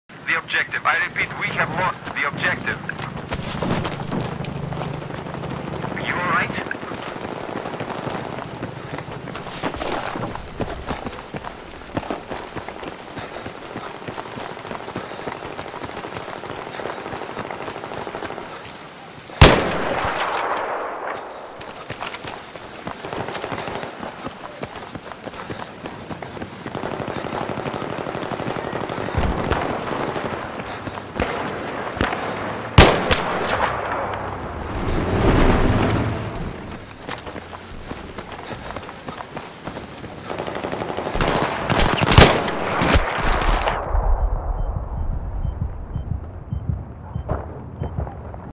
Fraps setup quality test
Half Screen, 30fps capture, also ran it through Handbrake;
file went from 500mb to just under 40mb :)